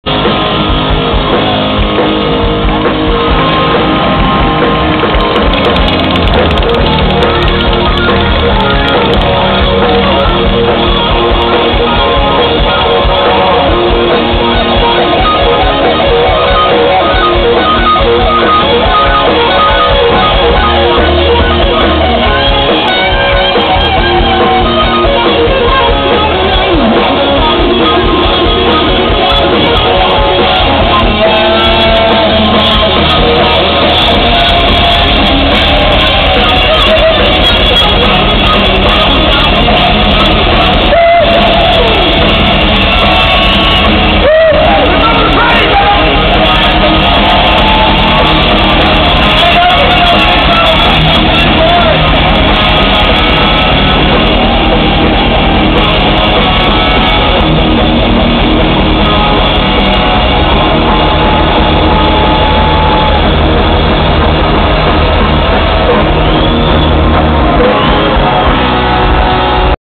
Dam Bar & Grill, Lake Murray SC, Bike Party
DBG bike party, June 2007. Late crowd shot, panoramic view.